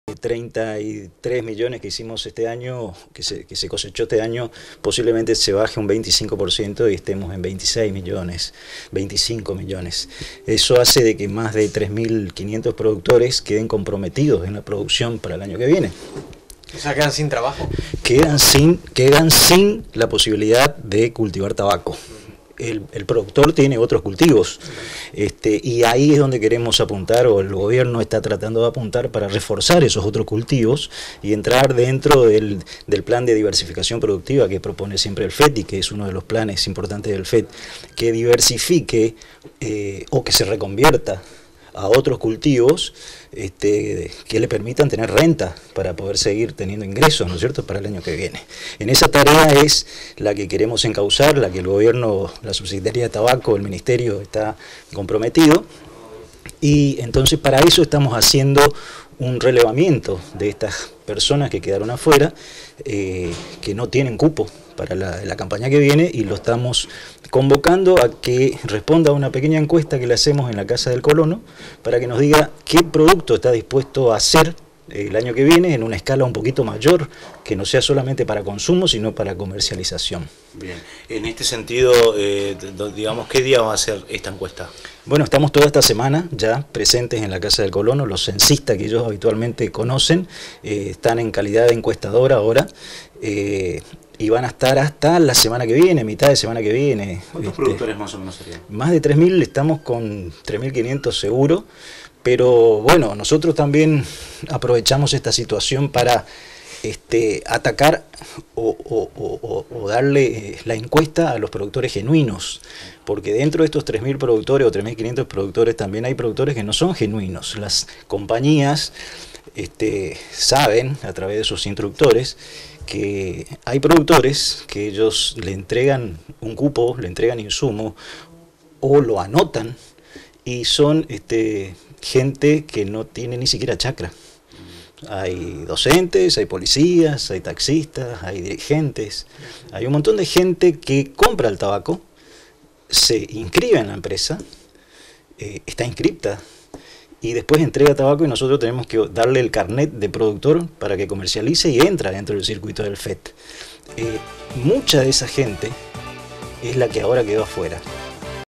Encuesta para tabacaleros
Realizarán encuesta para organizar a productores tabacaleros.